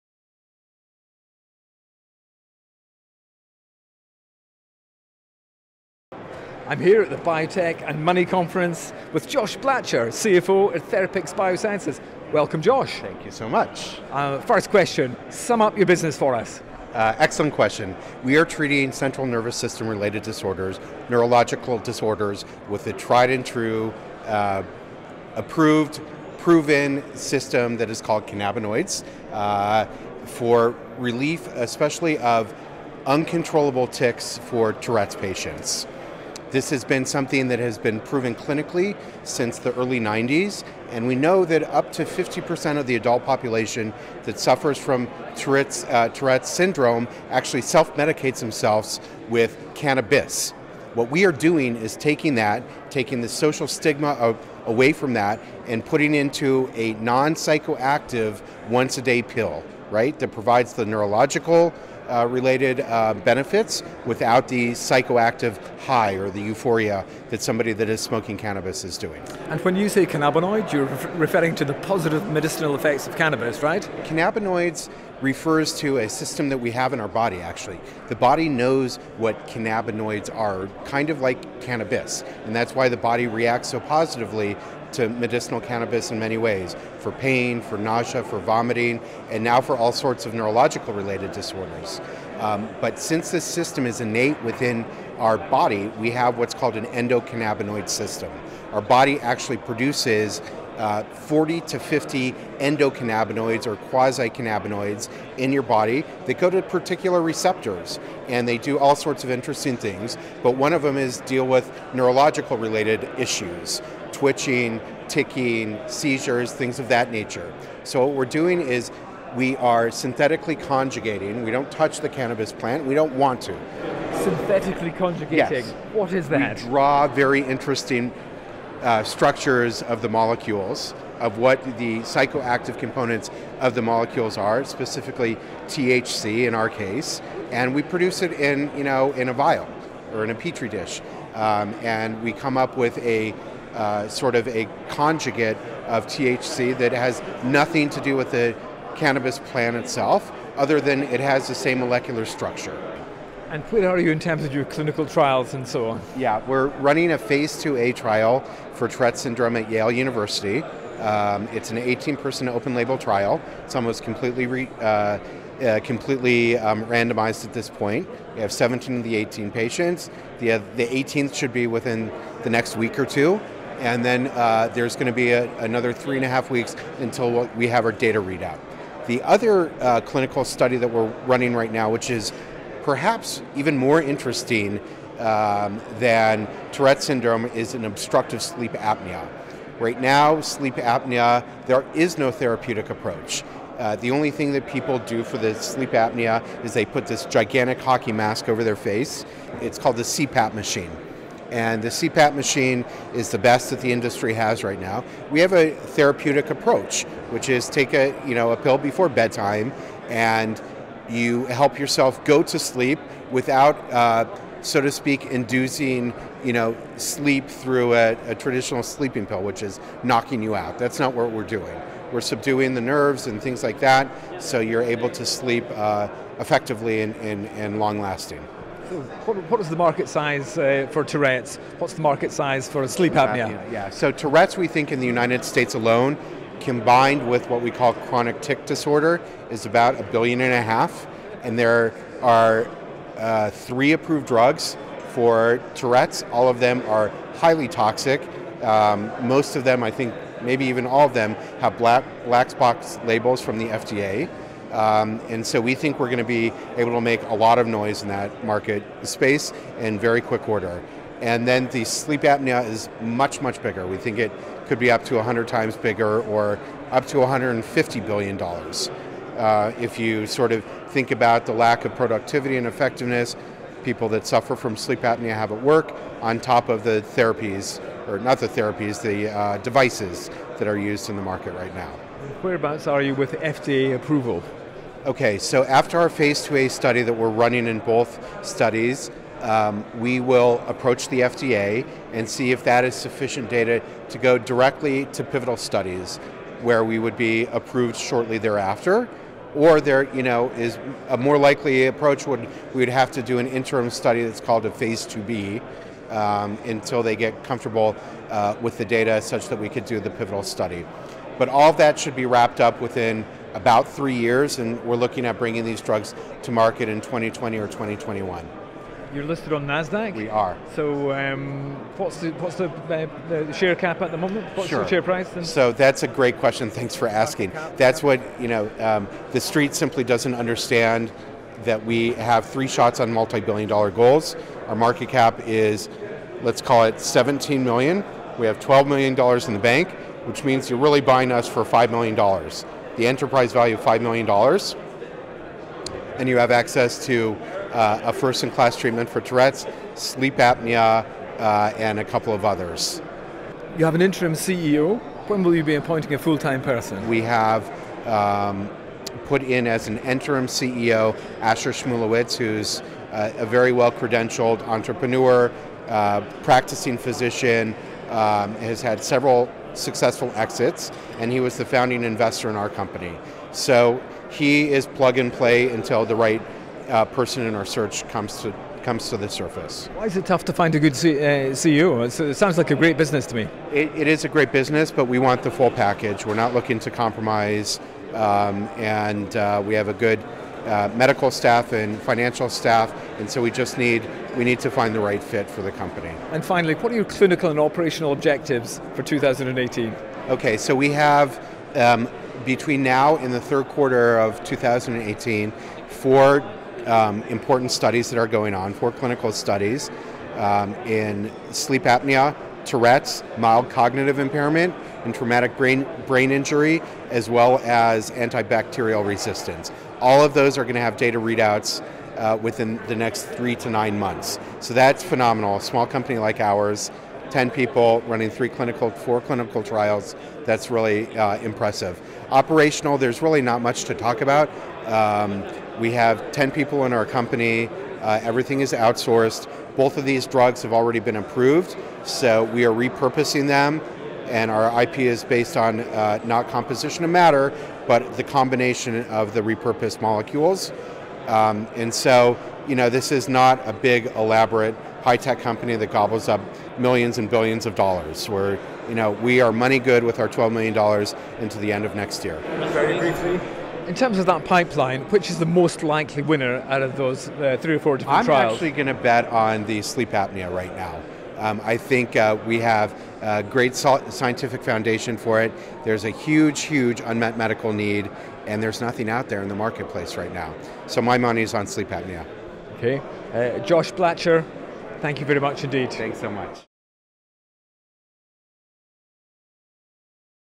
PHARMA: Therapix Biosciences are developing 'medicinal cannabis' for Tourette's and sleep apnea
Josh Blacher, CFO at Therapix Biosciences, describes how his company are developing a therapeutic approach to obstructive sleep apnea, and in parallel are developing a synthetic cannabinoid-based tablet to treat Tourette's Syndrome.

What are cannabinoids?

Cannabinoids (e.g., THC and CBD) are the chemical compounds secreted by cannabis flowers that provide relief to an array of symptoms including pain, nausea, anxiety, and inflammation. These work by imitating compounds our bodies naturally produce, called endocannabinoids, which activate to maintain internal stability and health. To put a complex system simply, they mediate communication between cells, and when there is a deficiency or problem with our endocannabinoid system, unpleasant symptoms and physical complications occur.

Therapix are running a Phase IIa clinical trail for Tourette's Syndrome Yale at Yale University with data due in a month or so, and also a Phase IIa study for sleep apnea.

According to Therapix, the market potential for sleep apnea in the States is up to 150 billion dollars, and for Tourette's it is 1.5 billion dollars, and Therapix hope to bring tablet-based therapies to market in 2020 or 2021.

Therapix are NASDAQ-listed: TRPX

This interview is one of a series with Edison pharma clients shot at the Biotech and Money Conference at the Waldorf Hotel, London on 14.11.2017.